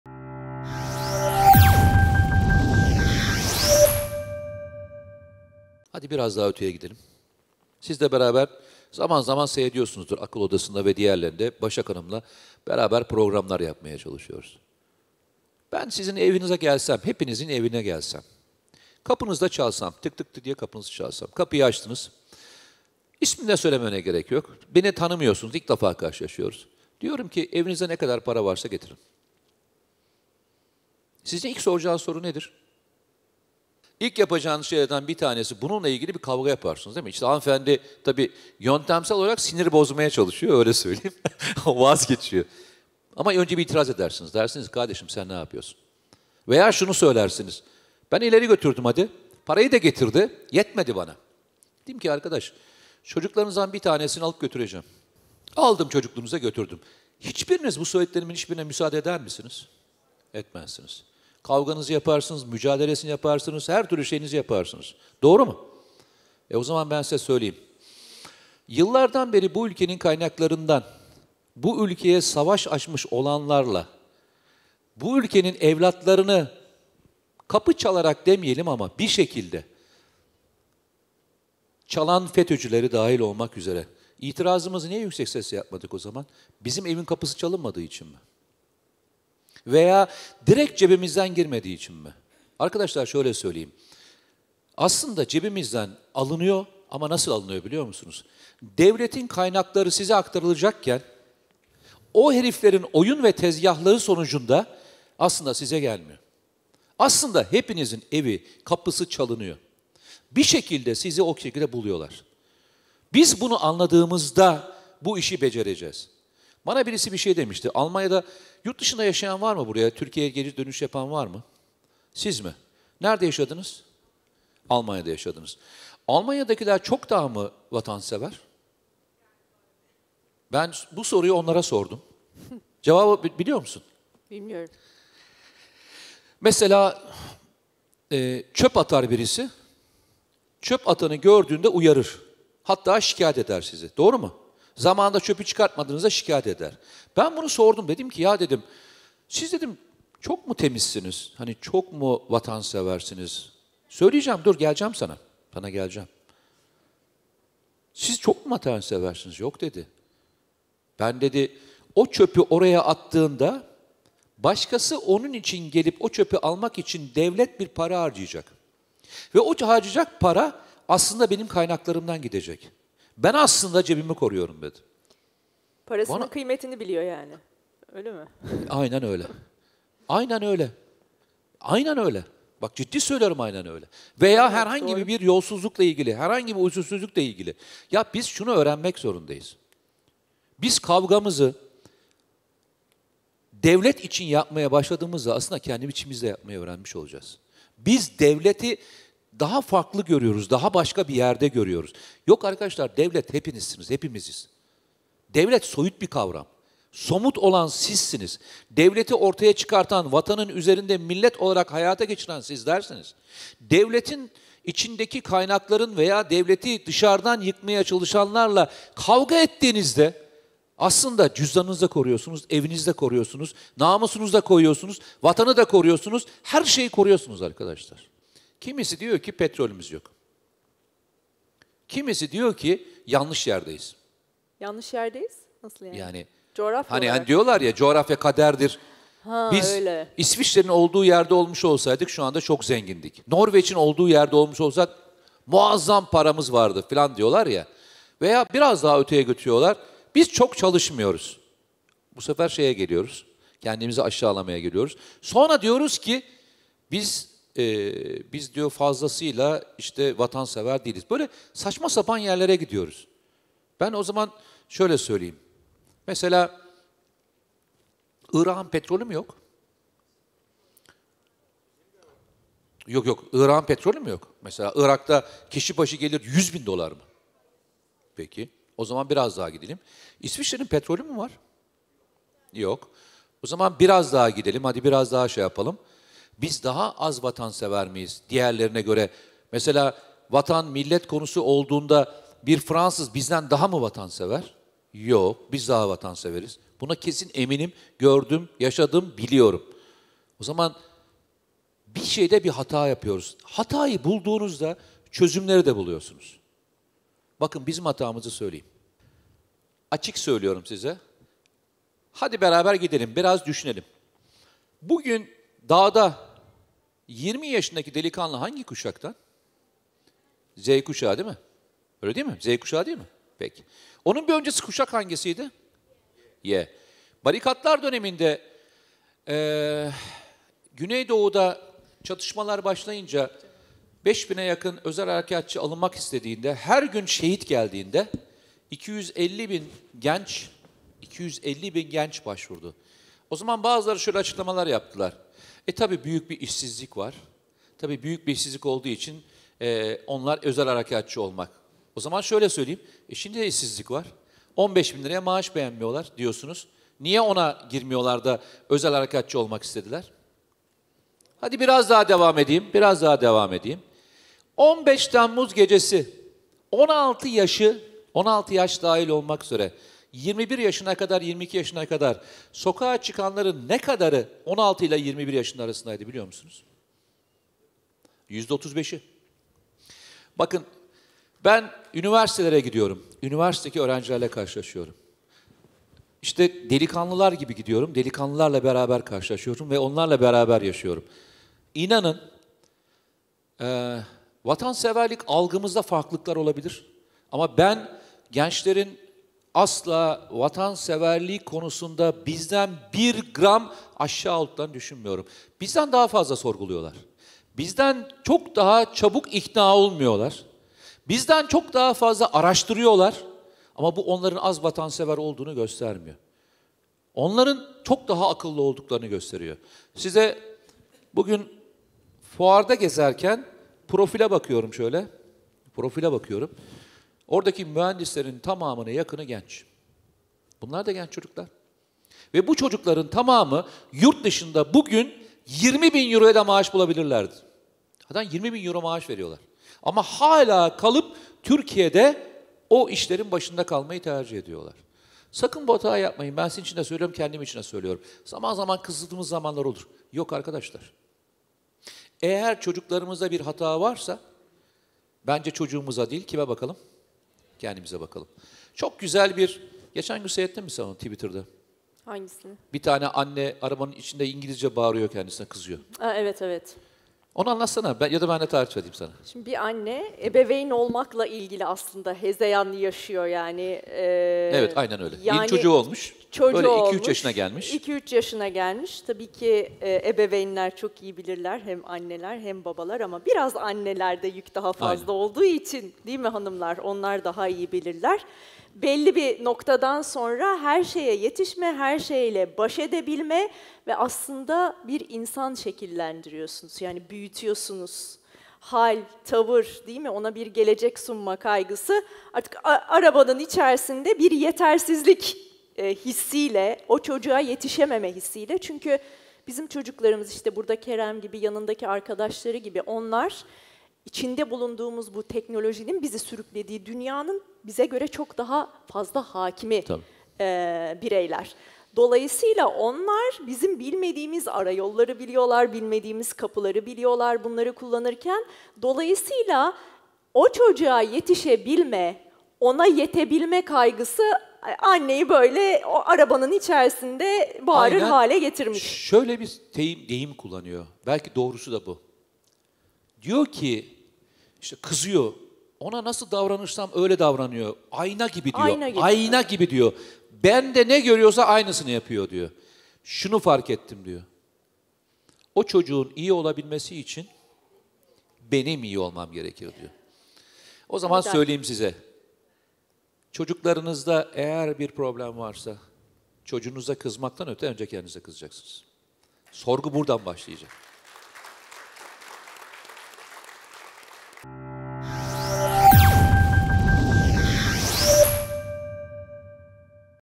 Mete Yarar "Çocuğunun İyi Olmasını İstiyorsan Önce Sen İyi Olacaksın!"
Esenler Belediyesi, Evliya Çelebi Kültür Sanat Sezonu kapsamında gündeme dair son gelişmelerin ele alındığı “Düşünce Ekseni” adlı programı Esenlerlilerle buluşturdu. Başak Şengül’ün moderatörlüğünde her ay meraklılarıyla buluşacak olan programın ilk konuğu Güvenlik Politikaları Uzmanı Mete Yarar oldu.